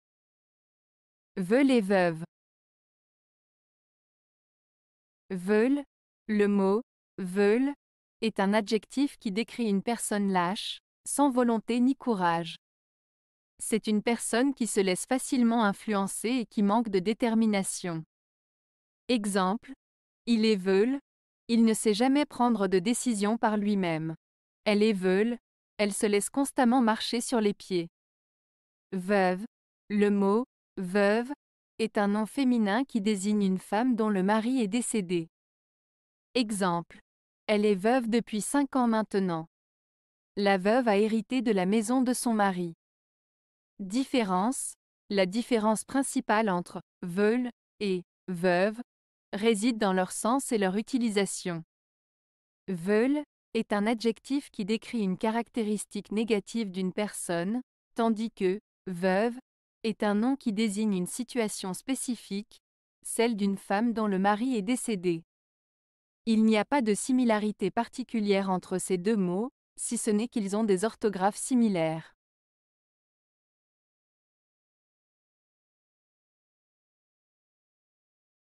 Veule et Veuve (Une Lettre)
Veule:
Le mot "veule" est un adjectif qui décrit une personne lâche, sans volonté ni courage. C'est une personne qui se laisse facilement influencer et qui manque de détermination.

Exemples:
- Il est veule, il ne sait jamais prendre de décisions par lui-même.
- Elle est veule, elle se laisse constamment marcher sur les pieds.

Veuve:
Le mot "veuve" est un nom féminin qui désigne une femme dont le mari est décédé.

Exemples:
- Elle est veuve depuis cinq ans maintenant.
- La veuve a hérité de la maison de son mari.

Différence:

La différence principale entre "veule" et "veuve" réside dans leur sens et leur utilisation. "Veule" est un adjectif qui décrit une caractéristique négative d'une personne, tandis que "veuve" est un nom qui désigne une situation spécifique, celle d'une femme dont le mari est décédé. Il n'y a pas de similarités particulières entre ces deux mots, si ce n'est qu'ils ont des orthographes similaires.